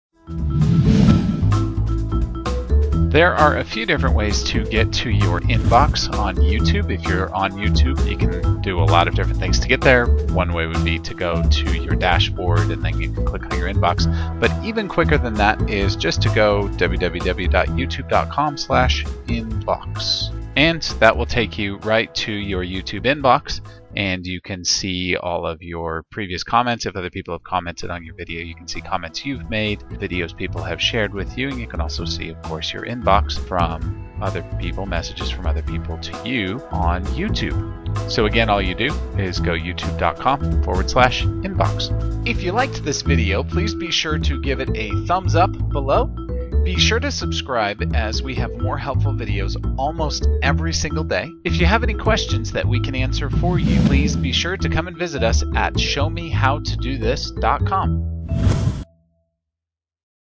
How to View Your Youtube Inbox
Getting to your inbox on YouTube is simple.

Just add the /inbox after the YouTube URL.

In fact you should be able to click that link above, and get to your inbox, if you are logged in to YouTube at the moment.